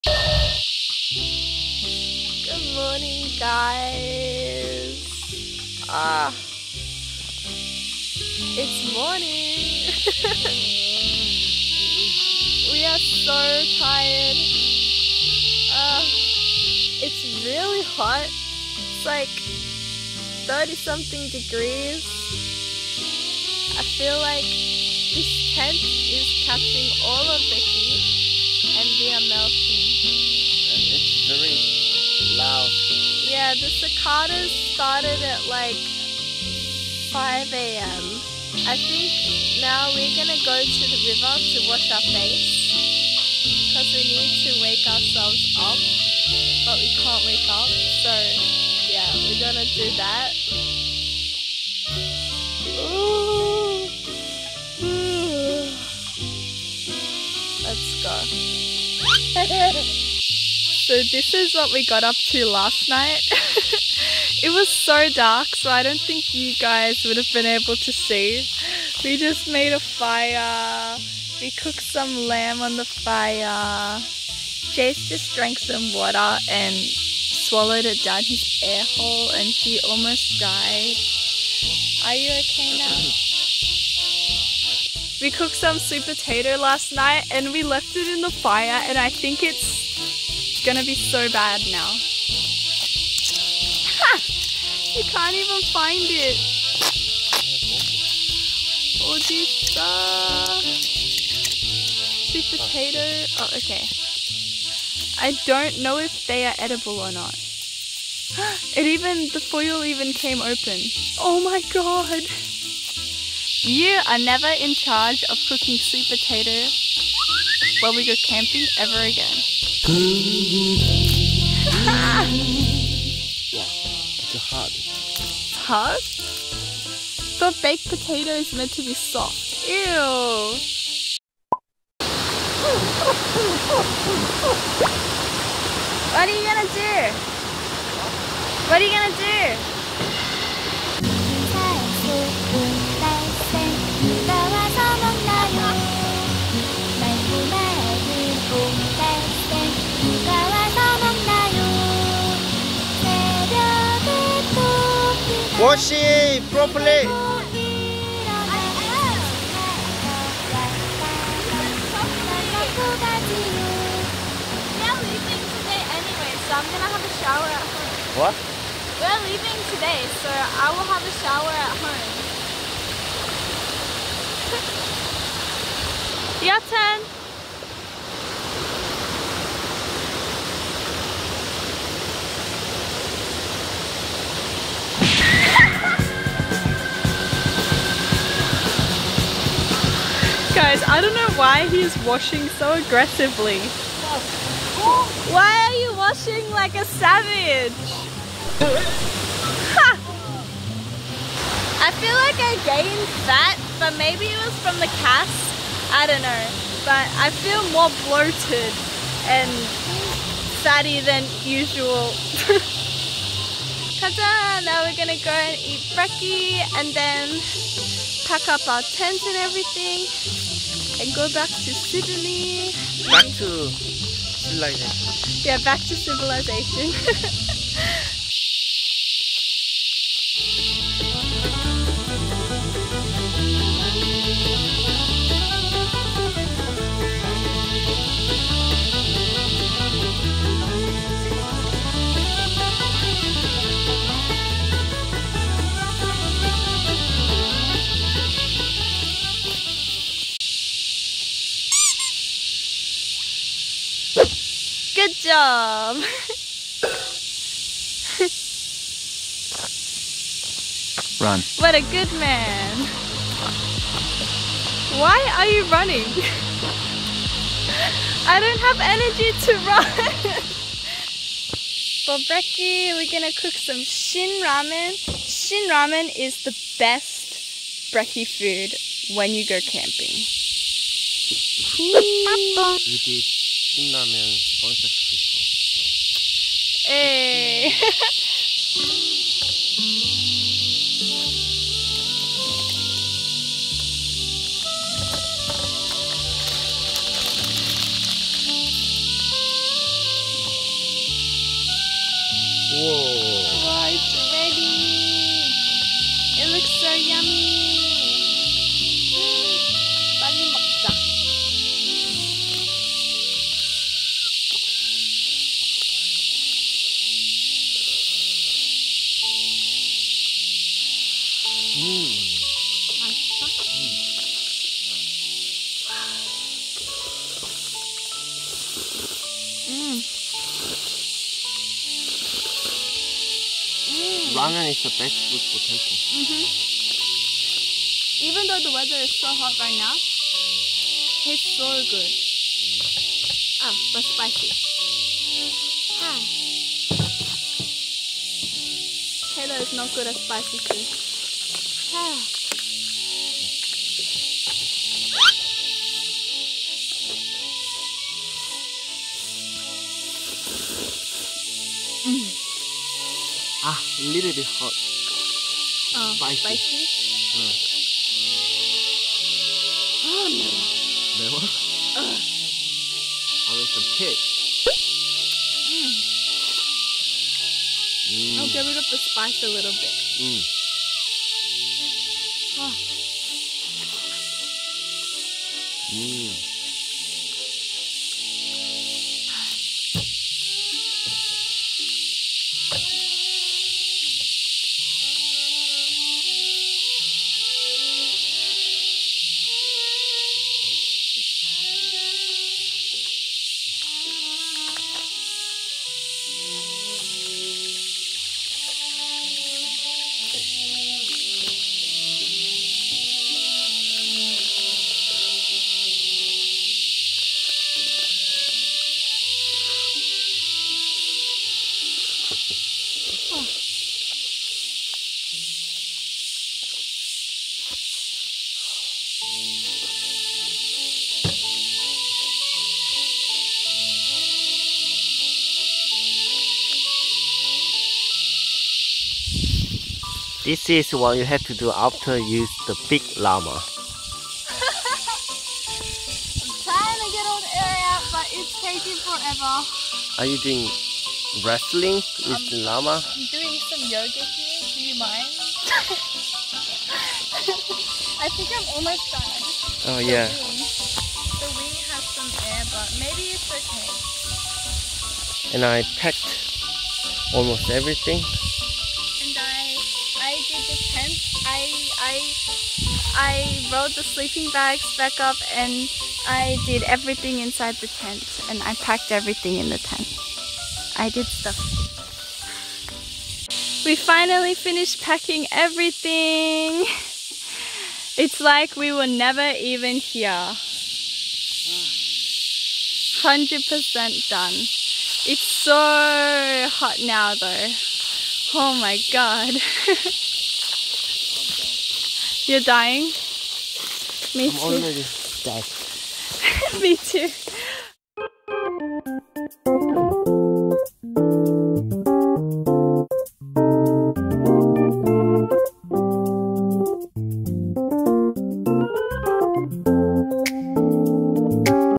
Living In The Wilderness Day To Day Lifestyle | VLOG Camping Morning Routine! | AMWF COUPLE
And follow our Instagram as well^^